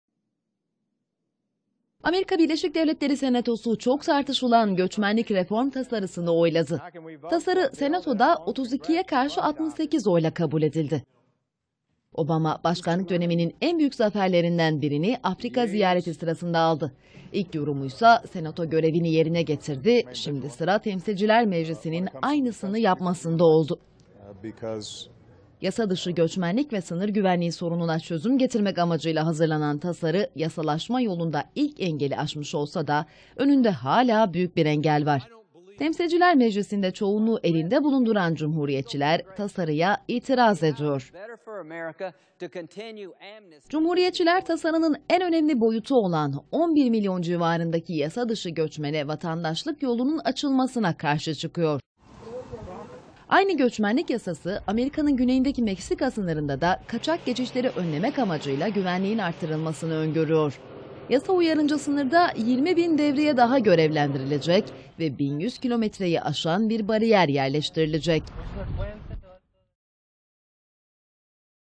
ABD Senatosu yeni Göç Yasası'nı onayladı
ABD Senatosu 11 milyon kaçak göçmene vatandaşlık yolunu açabilecek geniş kapsamlı göç yasa tasarısını onayladı. Obama haberi Afrika ziyareti sırasında öğrendi. Amerikan başkanı "senato görevini yaptı" yorumunda bulundu